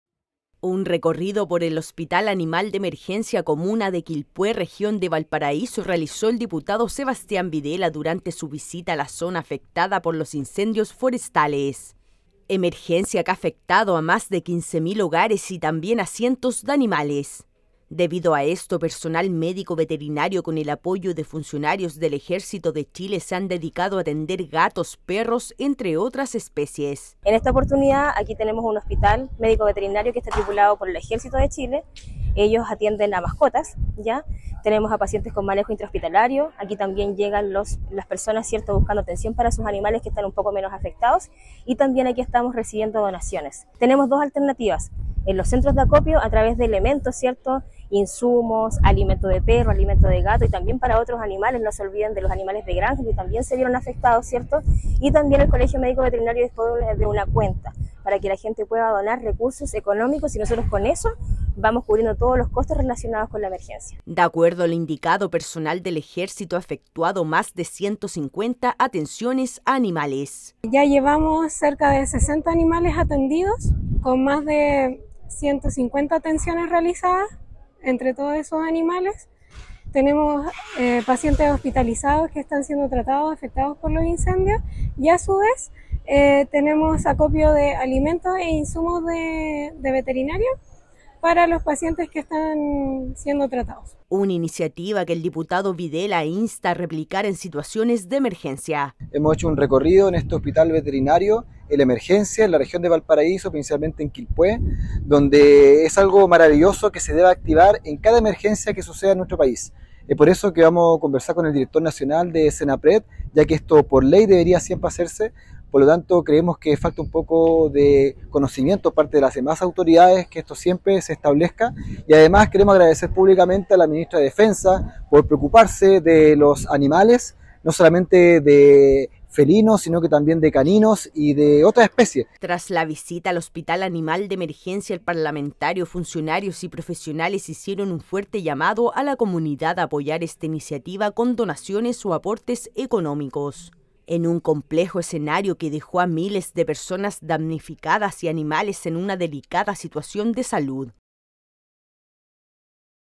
Animales afectados por incendios son atendidos en el Hospital Animal de Emergencia
Más de 100 atenciones se han realizado en el Hospital Animal de Emergencia, levantado en la comuna de Quilpué, región de Valparaíso, en el marco de los incendios forestales… La iniciativa es sostenida por personal médico veterinario y funcionarios del Ejército de Chile…. El diputado Sebastián Videla estuvo en el lugar y entregó detalles de este importante trabajo.